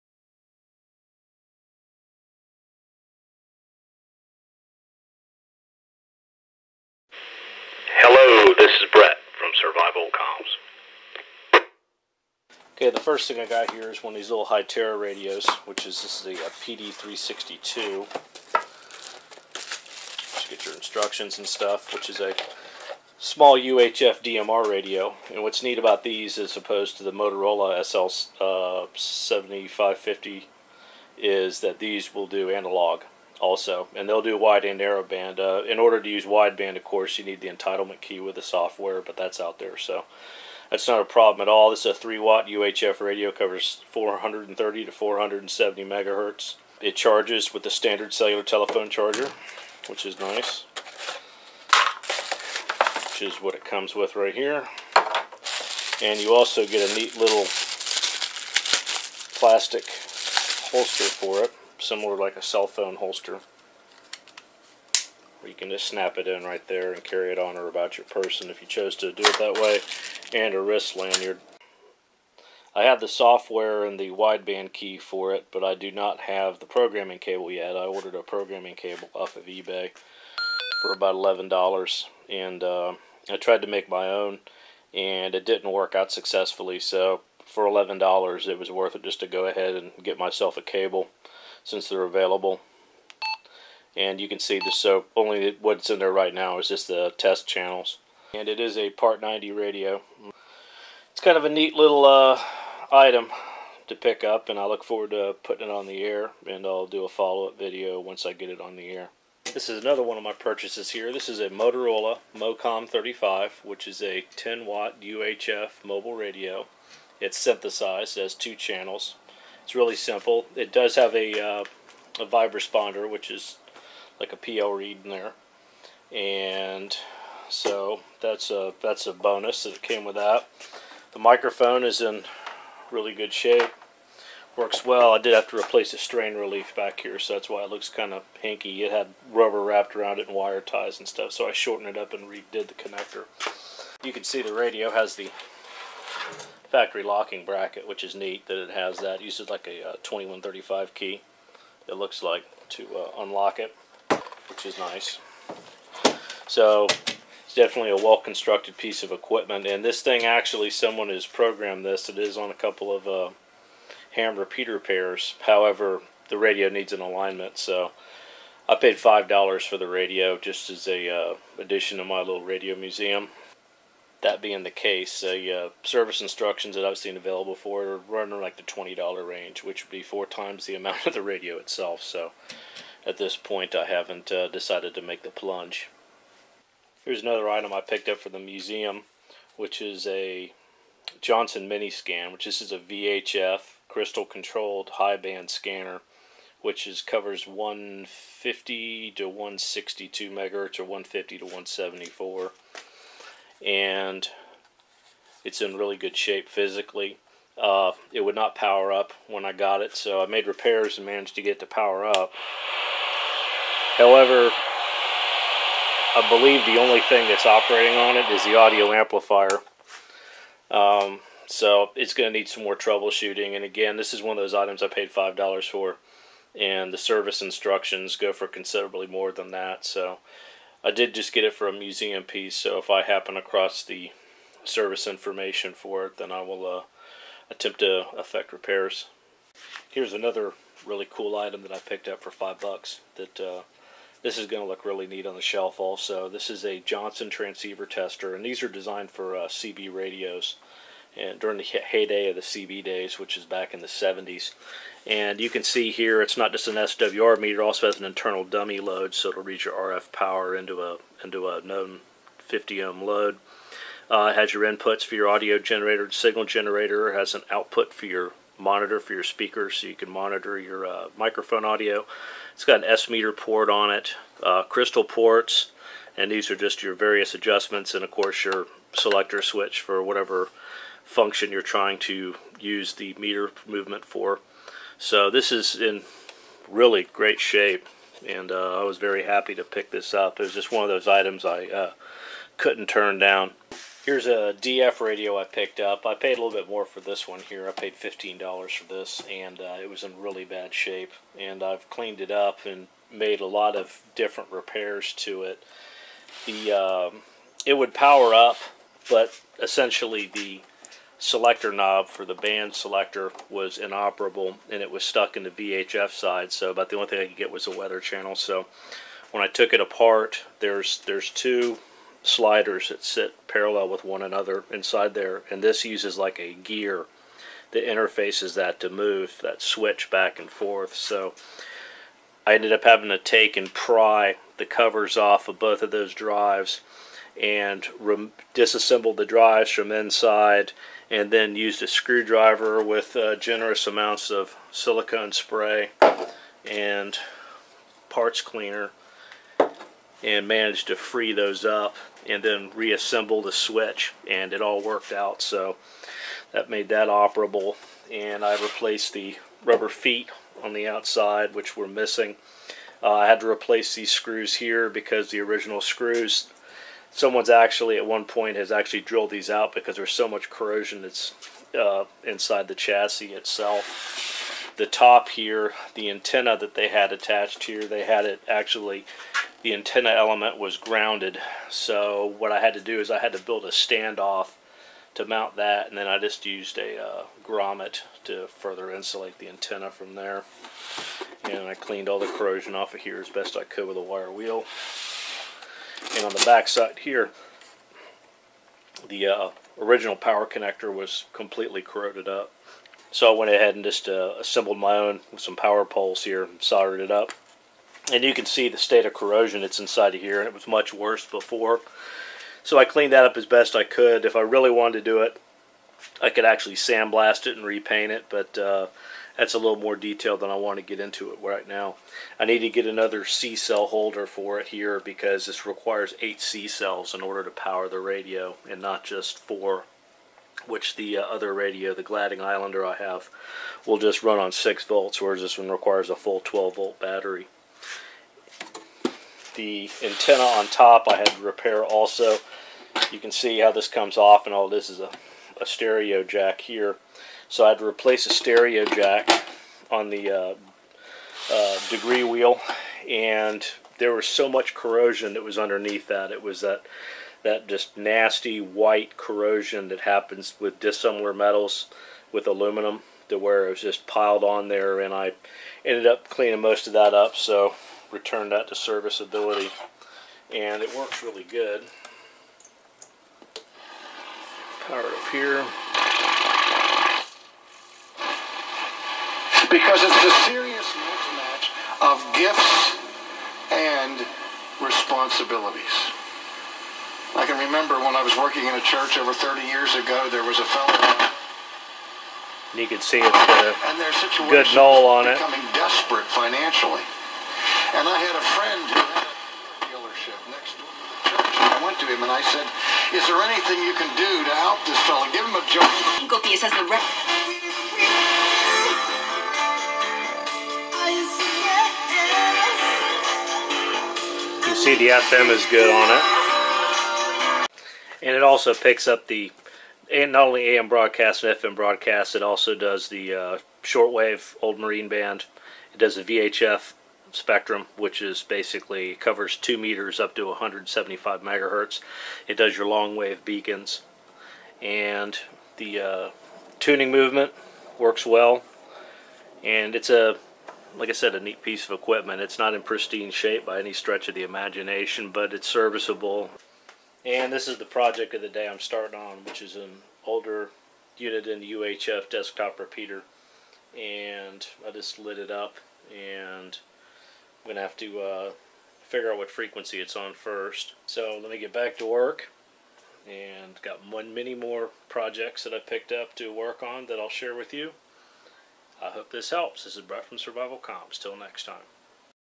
Hamcation 2018 radio projects part 1
Some of the stuff  I picked up at Hamcation 2018  that I have been working on this week. As I get more stuff squared away I will add more videos.